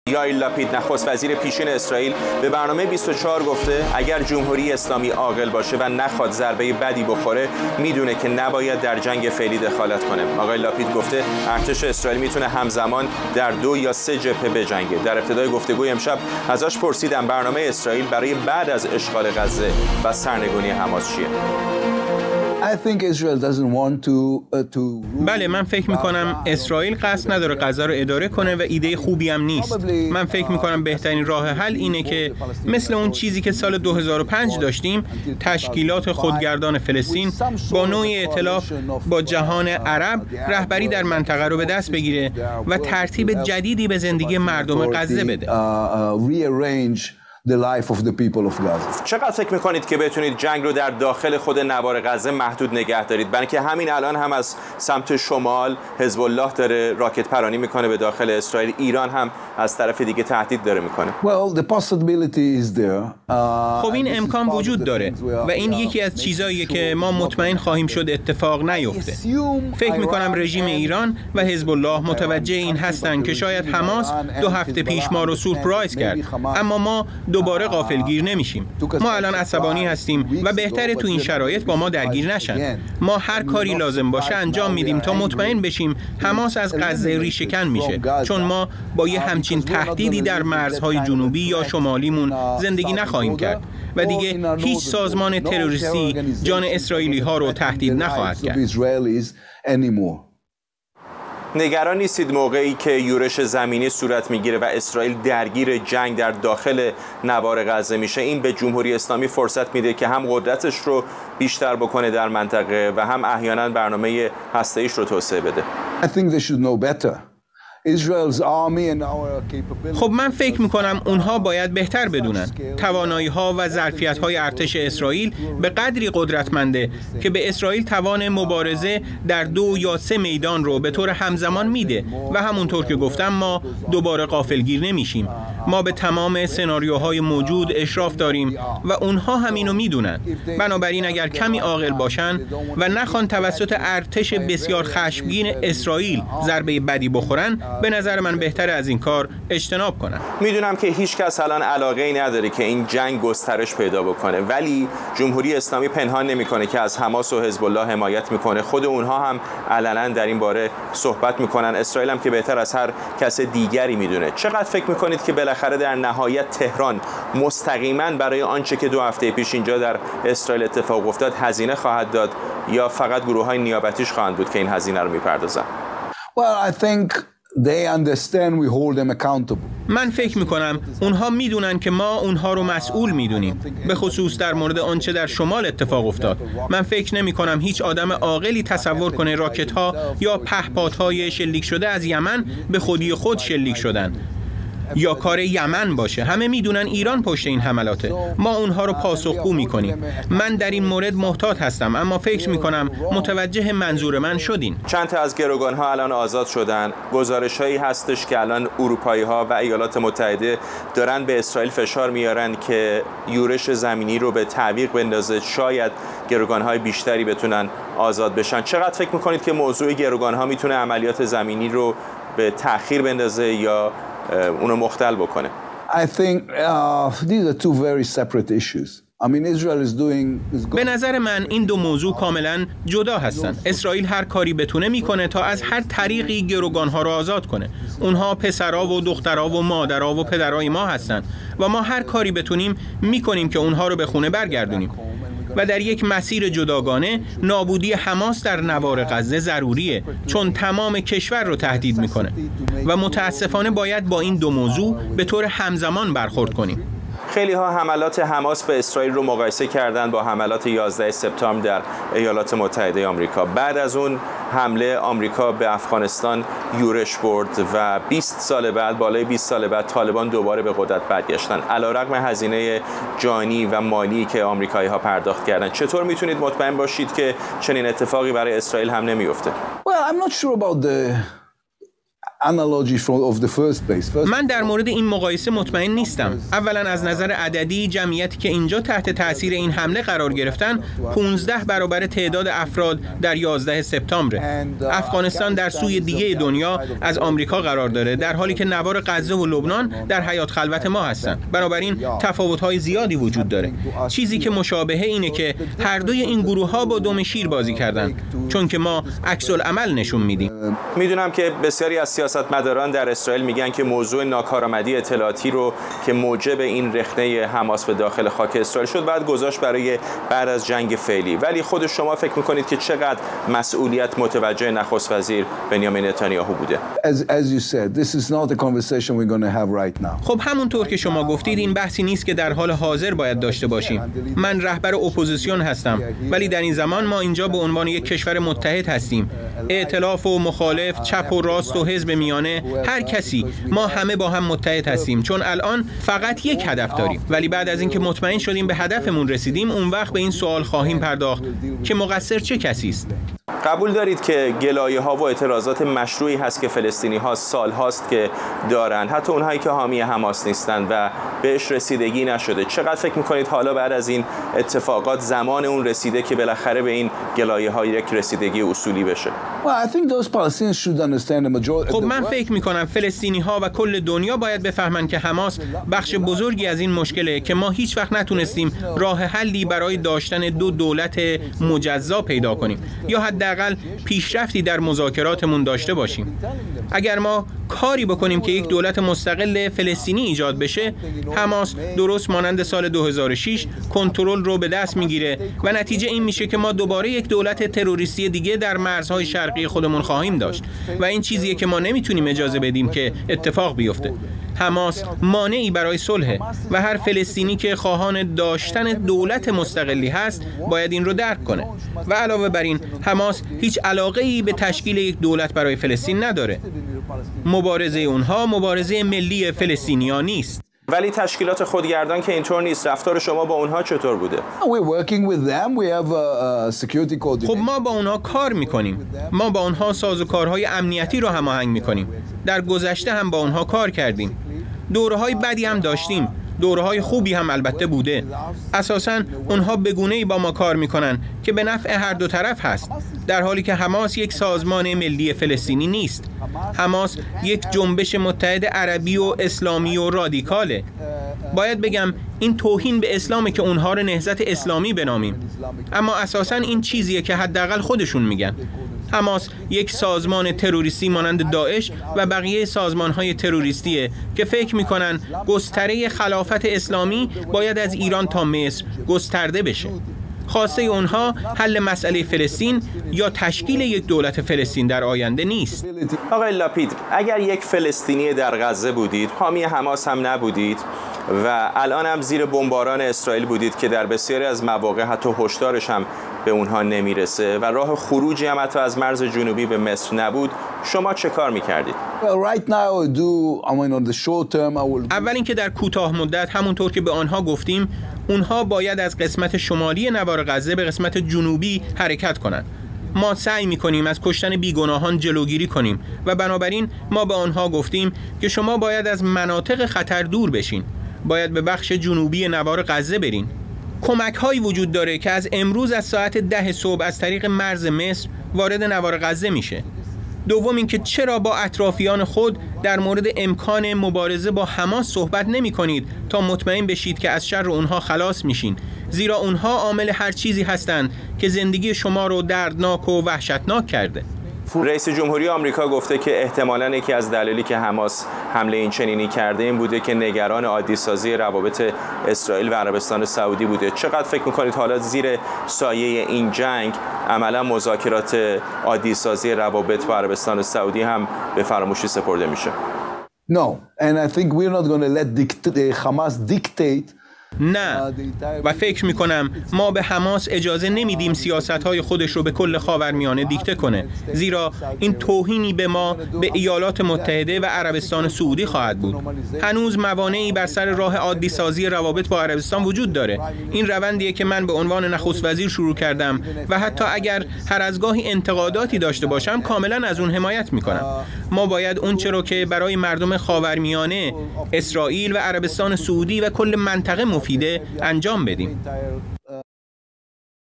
گفت‌وگو با یائیر لاپید، نخست وزیر پیشین اسرائیل
برنامه اسرائیل برای بعد از اشغال غزه و سرنگونی حماس چیست؟